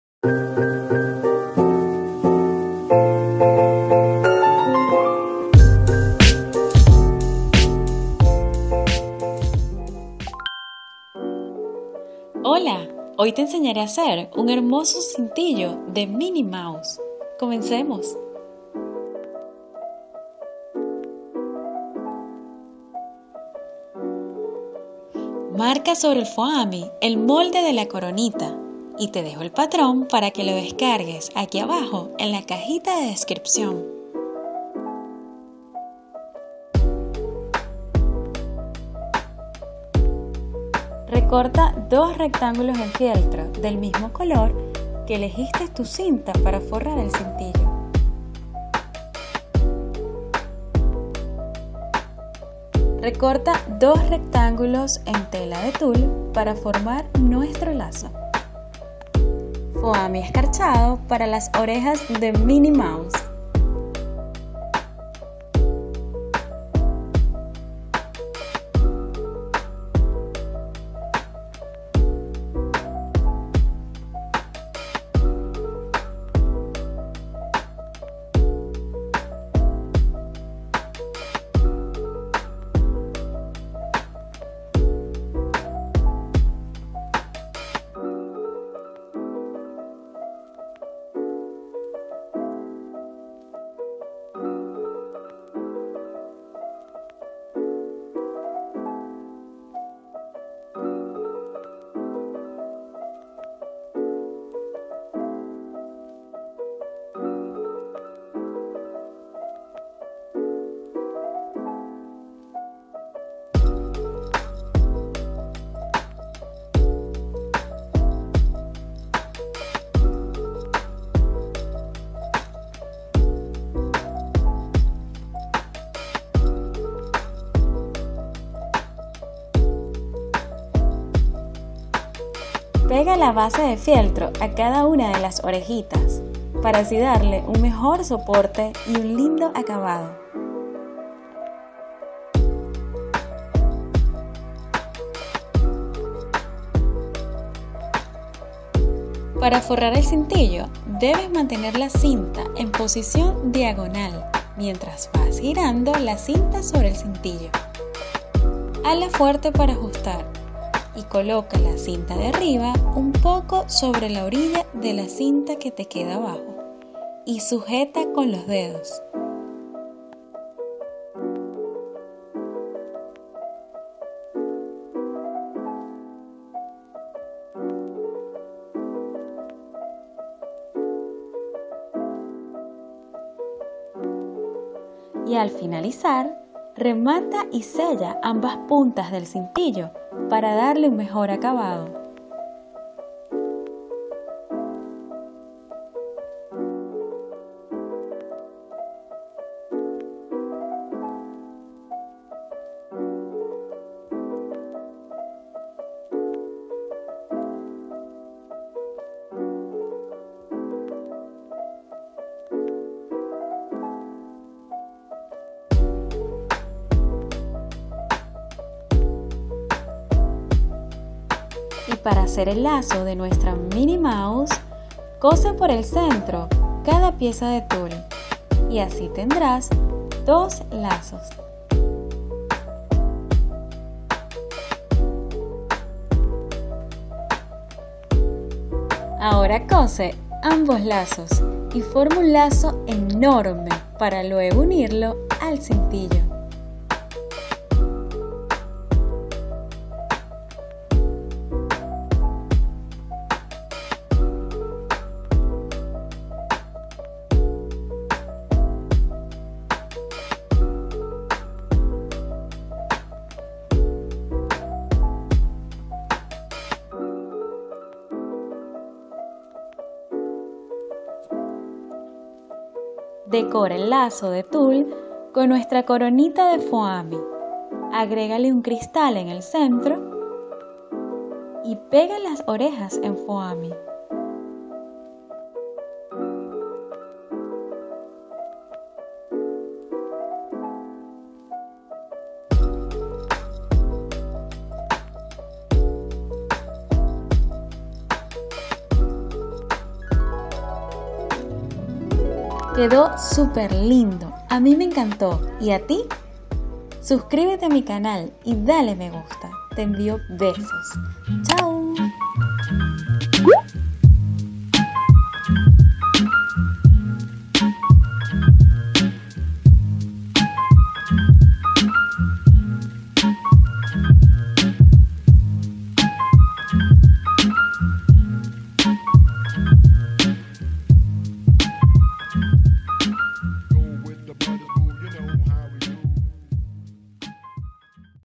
Cintillo Diadema Orejas Minnie Mouse FÁCIL- Foami - Lazo Tull/ - Headband -Hair Bow - Foam
Aprende a hacer este precioso Cintillo, con orejas de Minnie Mouse en Foami  y un elegante lazo en tela de Tull.

Agrega un toque mágico con la corona y el cristal, te dejo mas abajo el link para que descargues el molde de la corona .
¡Es  fácil de hacer! y queda muy lindo. Ideal para tu princesa consentida.